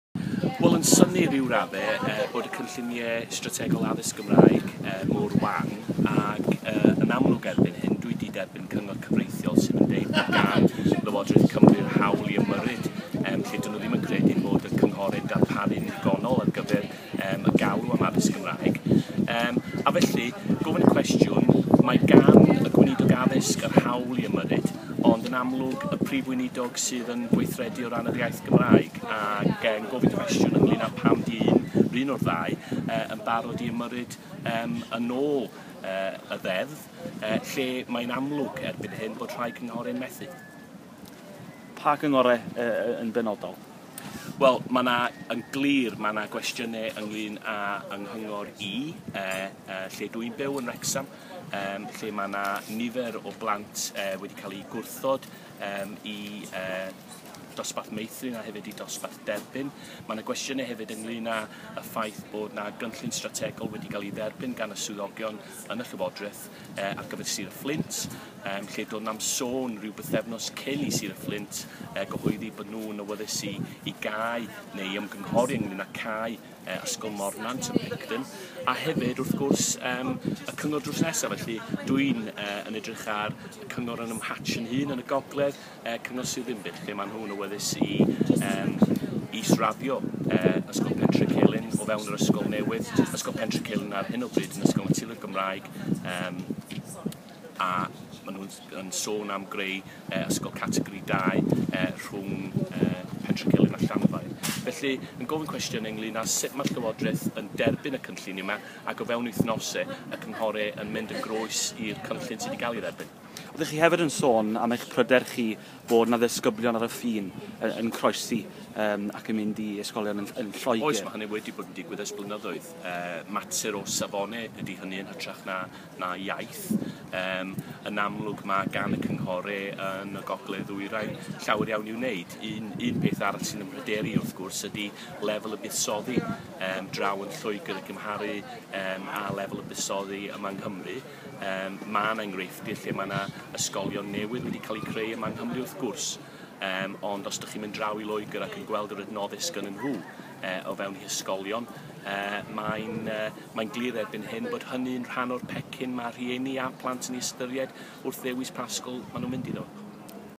Aled Roberts - addysg Gymraeg a Llywodraeth Cymru
Aled Roberts, llefarydd y Democratiaid Rhyddfrydol ar y Gymraeg, yn codi cwestiynau ynglŷn â chyfrifoldebau gweinidogion Llywodraeth Cymru dros yr iaith